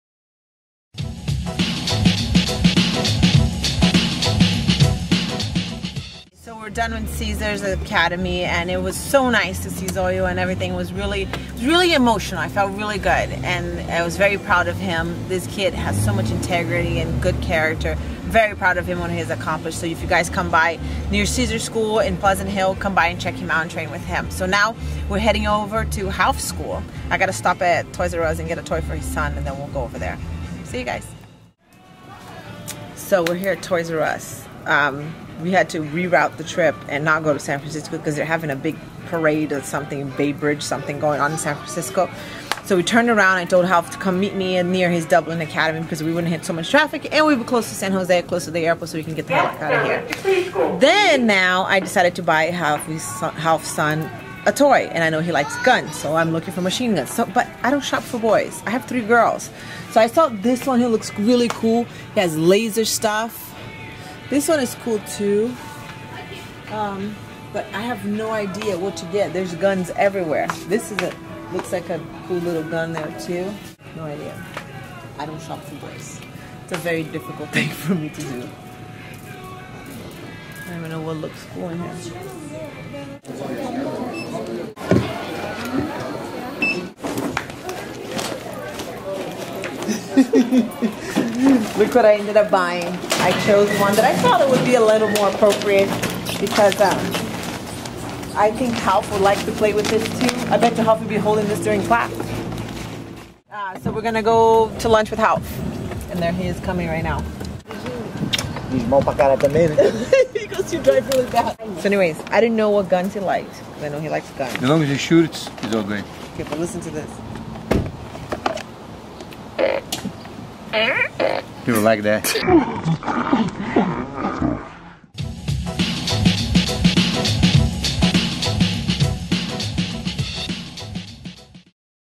Ralph Gracie & Rose Gracie - "The Gun"
After spending some quality time with Alessandro Ferreira at the Cesar Gracie Academy in Pleasant Hill, Ca., Rose Gracie heads out to meet Ralph Gracie for lunch, but first stops buy the store to buy a gun.